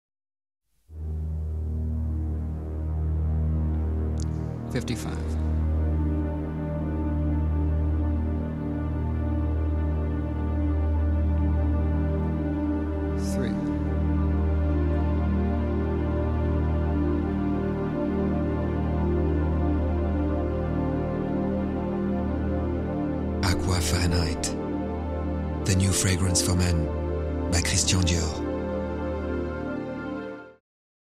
Christian Dior - Aqua Fahrenheit (Long Version) - 2011
Publicidade do novo perfume masculino da consagrada Maison Christian Dior .

Publicité de nouveau parfum pour homme de consagré Maison Christian Dior.
Le nouveau film Aqua Fahrenheit.